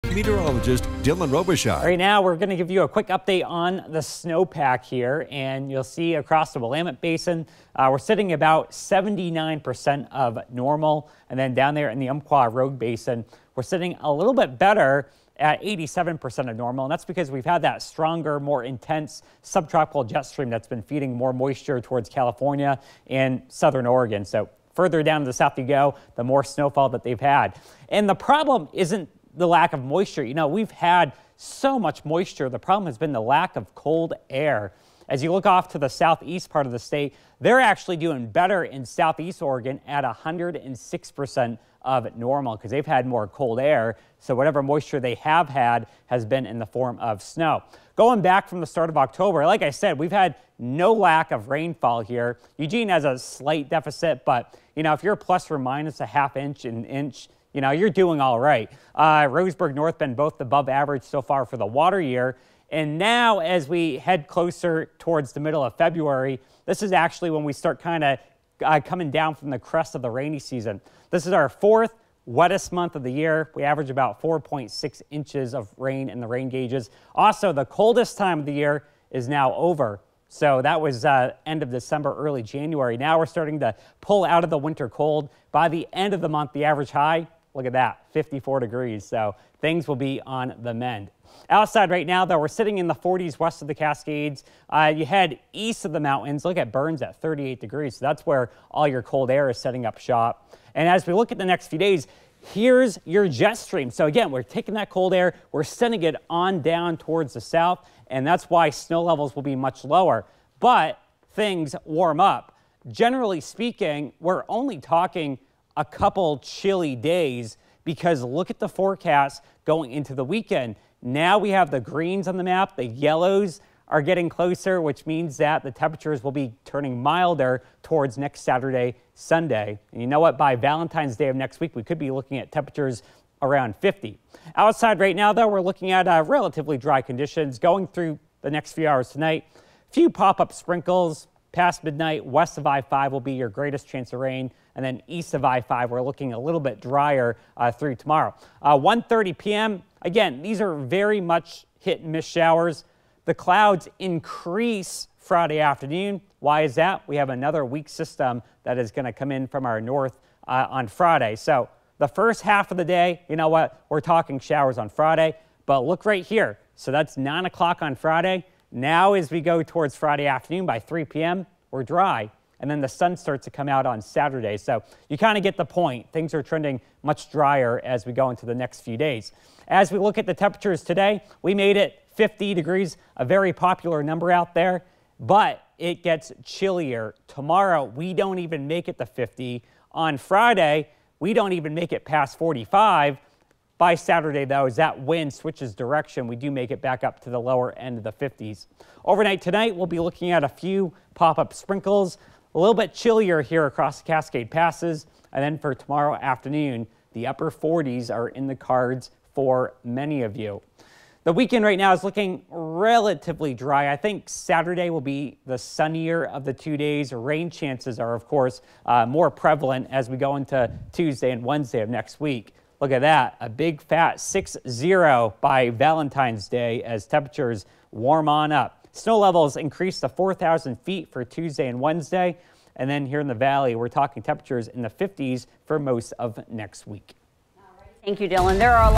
Here's an update on the snowpack across the Cascades
Despite there being plenty of precipitation, most of it fell in the form of rain, instead of snow, due to above average temperatures.